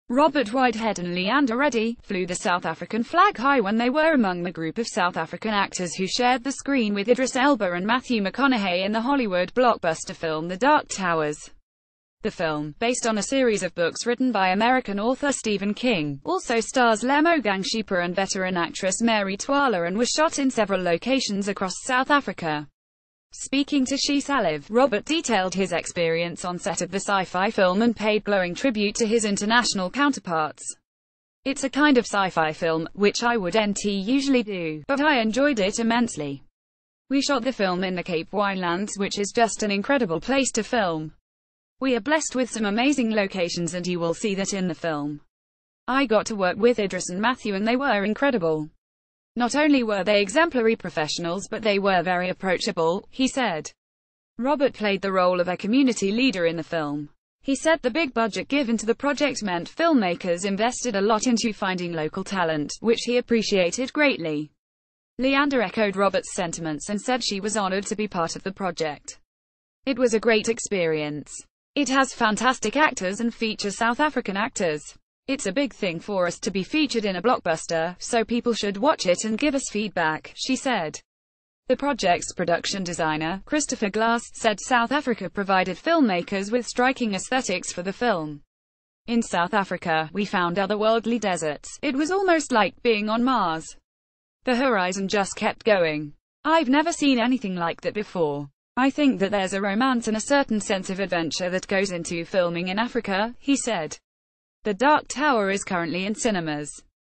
Robert whitehead, leeanda reddy shine in us blockbuster the dark tower | Africa Latest News
* Schedule: This channel posts new videos everyday!